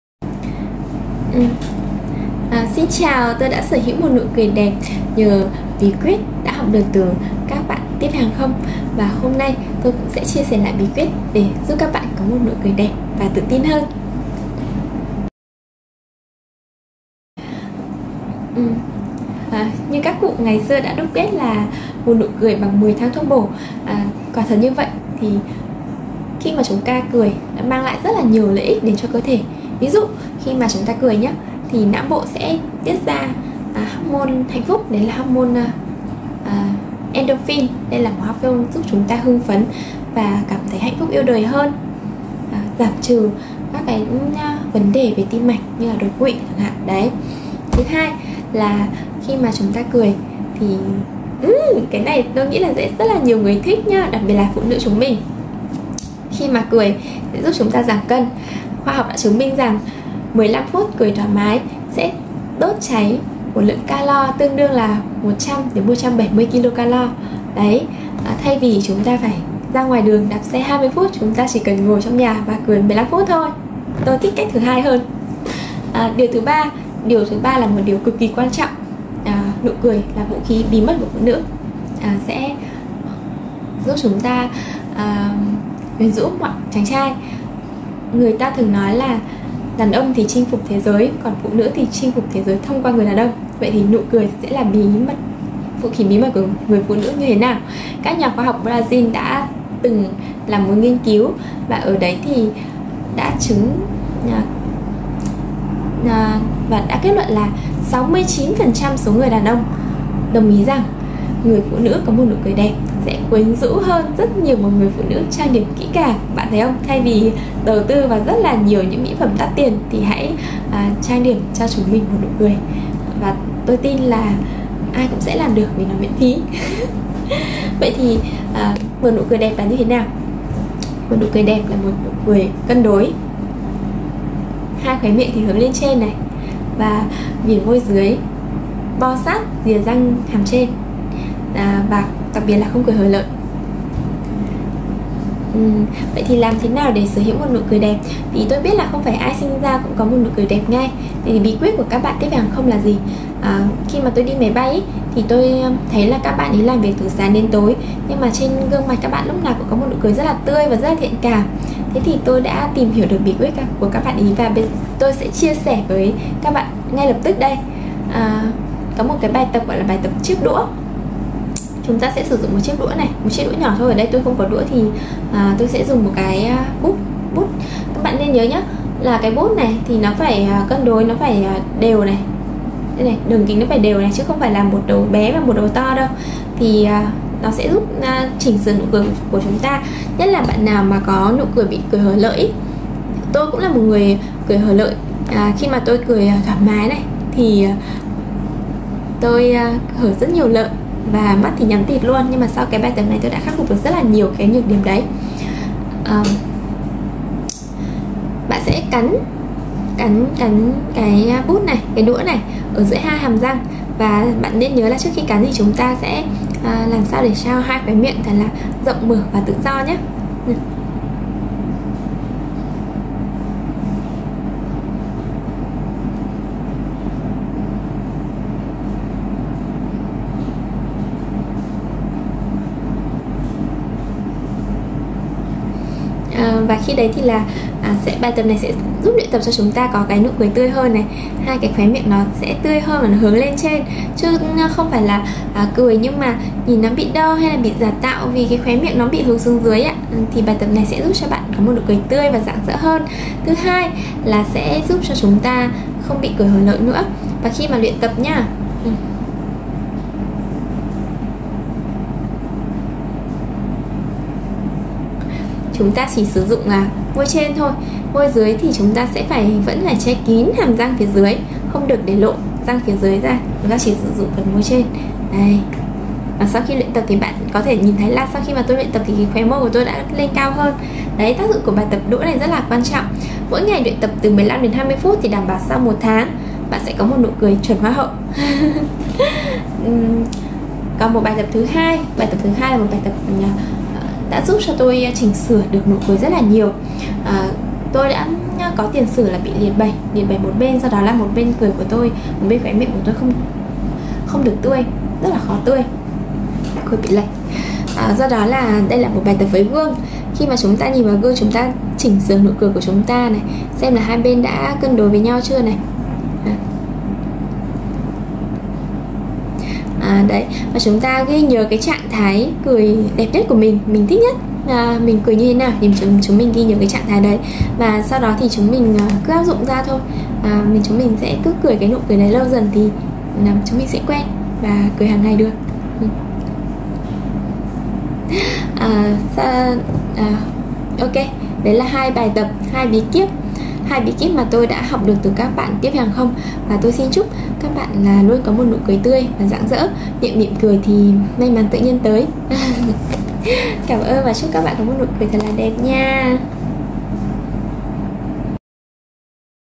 Tôi đã sở hữu nụ cười đẹp từ bí quyết của các Tiếp viên hàng không!
Một nụ cười đẹp mang đến cho bạn sự tự tin cũng như nhiều may mắn trong cuộc sống. Tuy nhiên, không phải ai cũng biết rằng nụ cười đẹp có thể luyện tập được. Trong video này bs Thu sẽ chia sẻ cho các bạn bí quyết có nụ cười đẹp như Tiếp viên hàng không nhé!